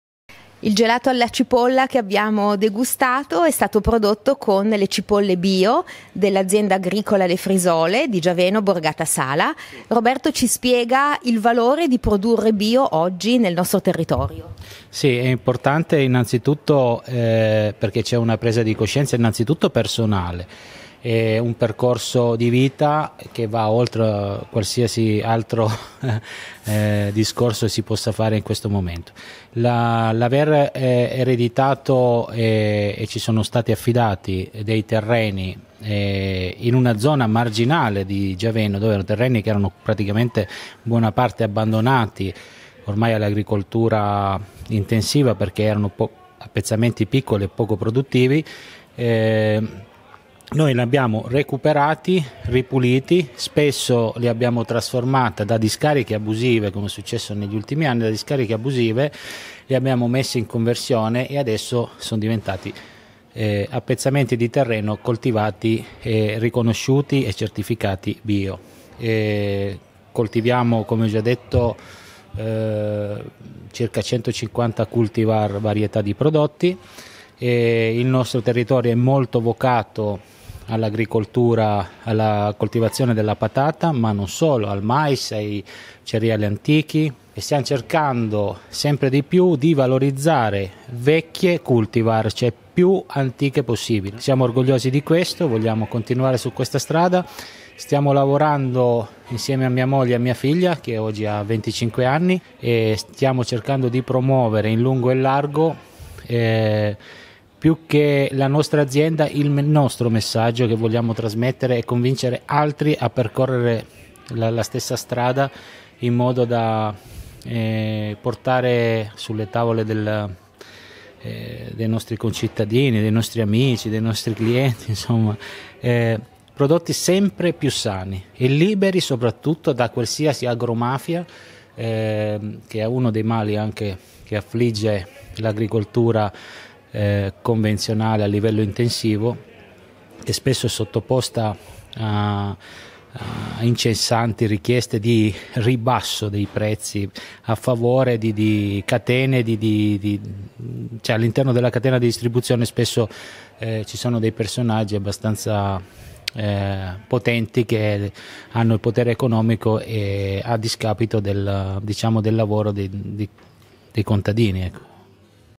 Agricoltura bio a Giaveno
Il valore di produrre bio sul nostro territorio, ne parla Roberto Castelli
dell'azienda agricole Le frisole di Giaveno